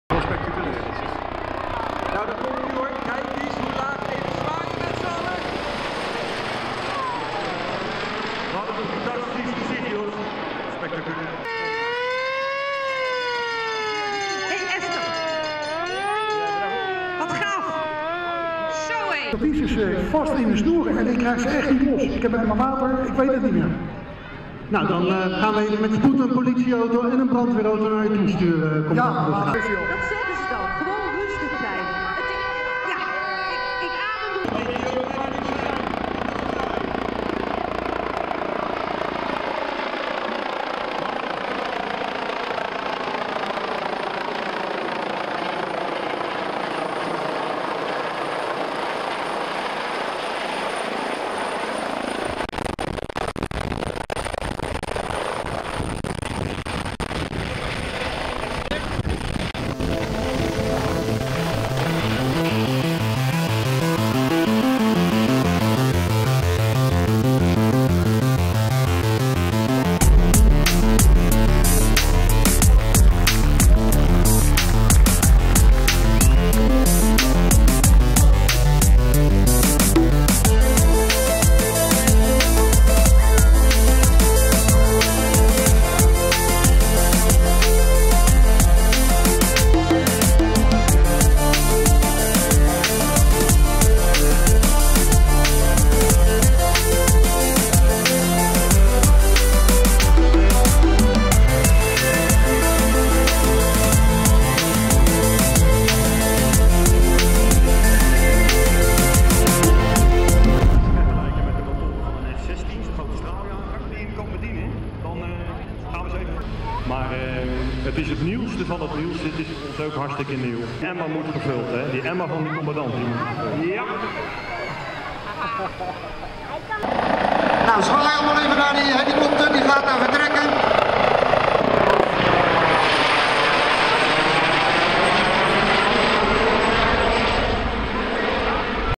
112 dag Den Haag 2015
Na een lange tijd van weg blijven werd dit jaar (2015) weer een 112 dag georganiseerd in het Zuiderpark in Den Haag. Hierbij een kleine impressie.